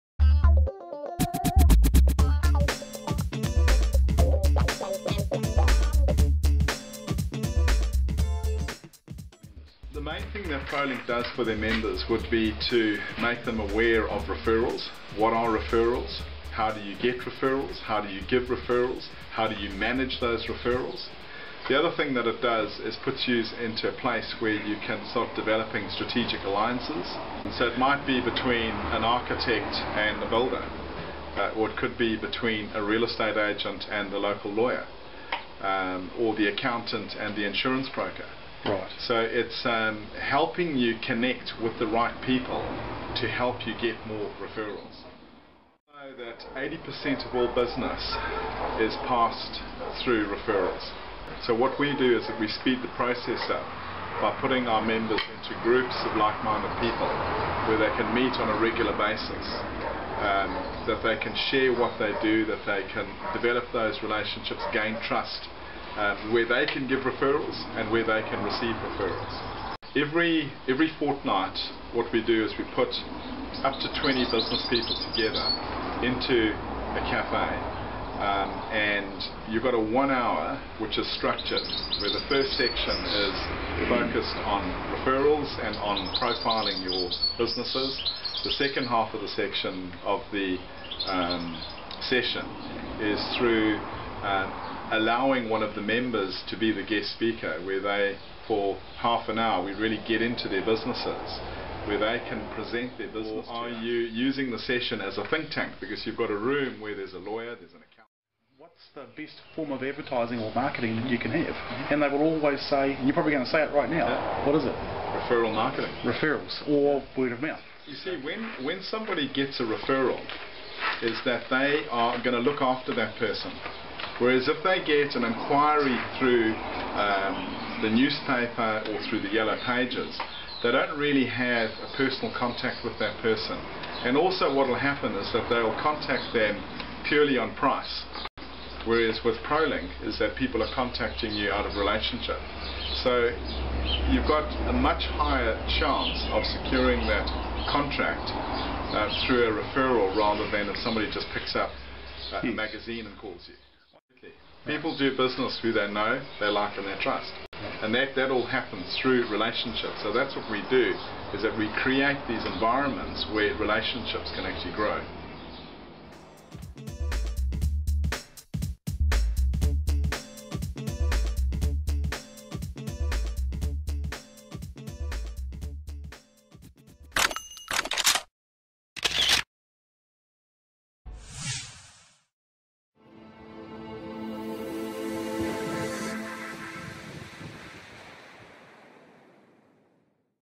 What is Prolink Central (an IMPROMPTU interview with owner Brett Yeatman)
Business to Business or Business to customer referrals